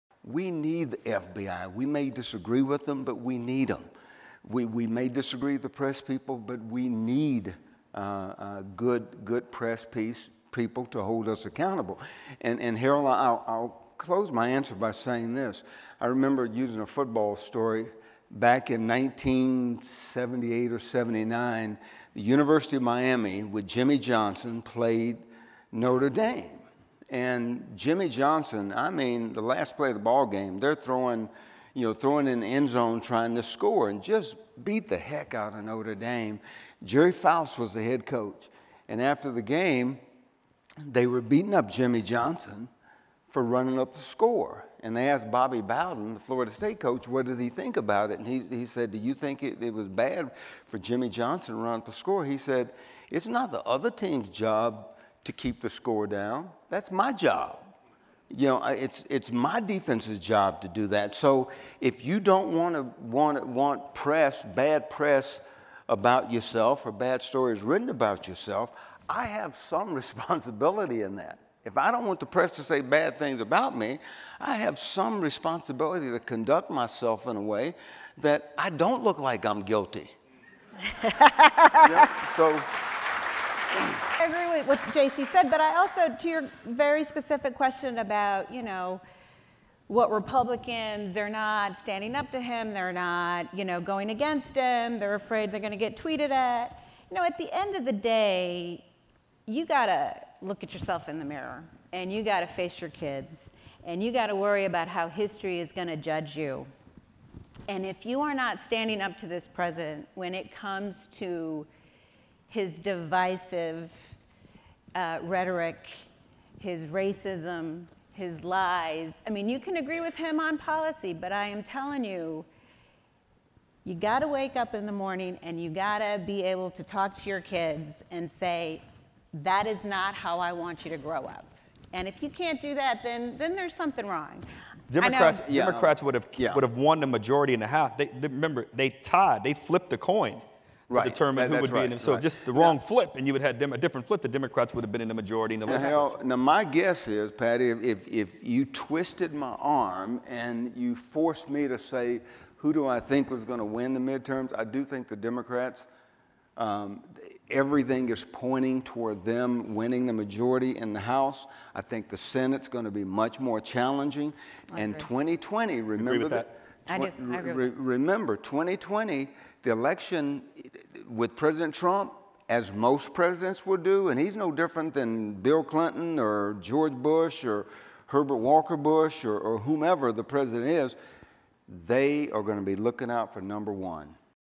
A Discussion with Patti Solis Doyle, Harold Ford Jr. and JC Watts Jr. | Mackinac Policy Conference
Harold Ford Jr. and Patti Solis Doyle joined JC Watts Jr. for a conversation about national politics, President Trump and the media during the 2018 Mackinac Policy Conference.